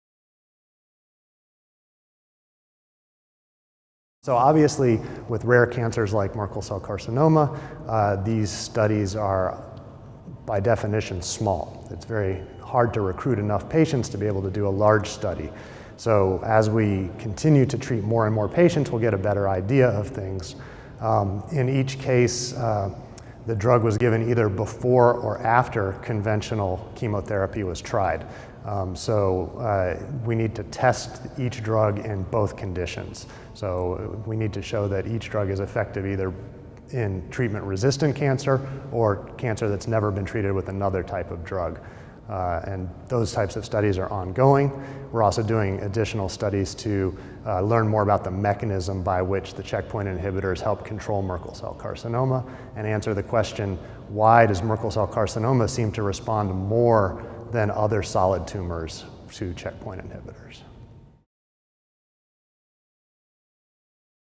What's next for Merkel cell carcinoma trials
Isaac Brownell, MD, PhD of the National Cancer Institute, Bethesda, MD discusses current studies into Merkel cell carcinoma while attending the 2016 World Congress on Cancers of the Skin (WCCS) and the Congress of the European Association of Dermato-Oncology (EADO) in Vienna, Austria. According to Dr Brownwell, with rare cancers like Merkel cell carcinoma, studies are typically small as it is difficult to recruit a large number of patients. Over time, as more patients are treated a better understanding can be achieved. In each trial, the drug was either given before or after conventional chemotherapy was tried, it is necessary to test the drug in both conditions to show whether the drugs work in treatment resistant cancer or cancer that has never been treated with another type of drug. These type of trials are ongoing as well as additional trials to learn more about the mechanism by which checkpoint inhibitors help control Merkel cell carcinoma and to answer the question – why does Merkel cell carcinoma seem to respond more than other solid tumors to checkpoint inhibitors?